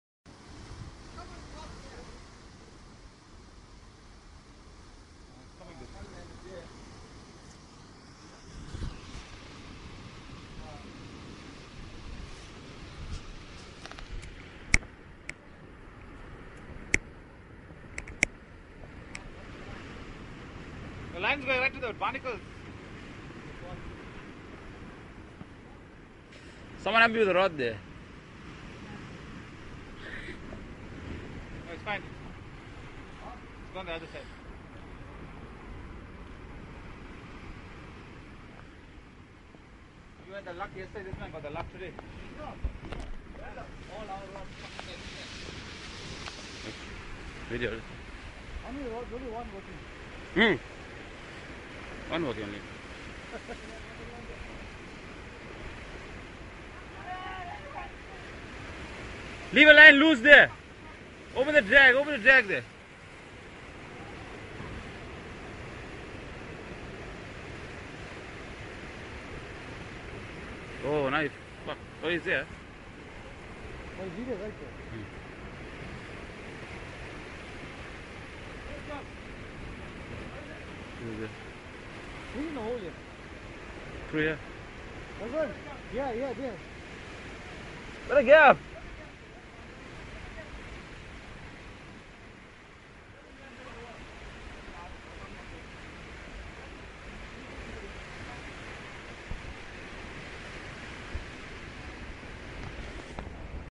Ronald Hammerhead Shark - Brazen Head Transkei
KZN Fishing went to Mpame, Transei in 2009 for their Premier League Finals. Ronald caught this shark, while fishing on the Saturday.